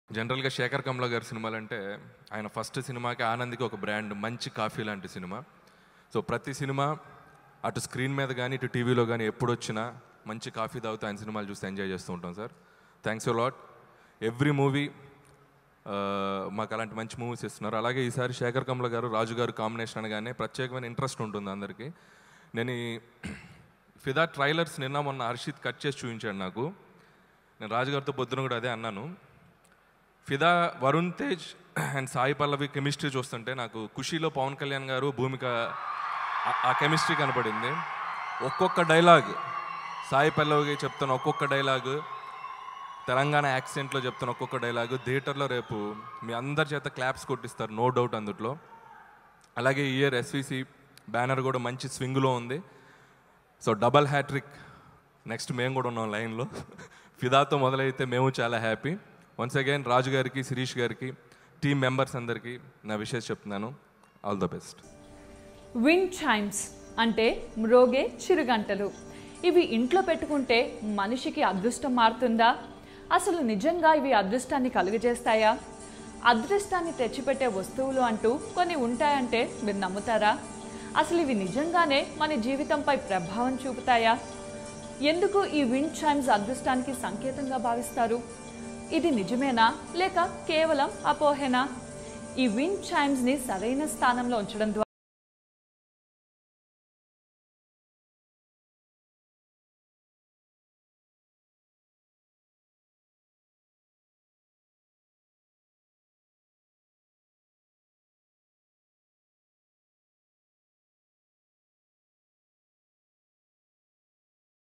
Anil Ravipudi Speech At Fidaa Movie Audio Launch || Varun Tej, Sai Pallavi
Watch Anil Ravipudi Speech At Fidaa Movie Audio Launch.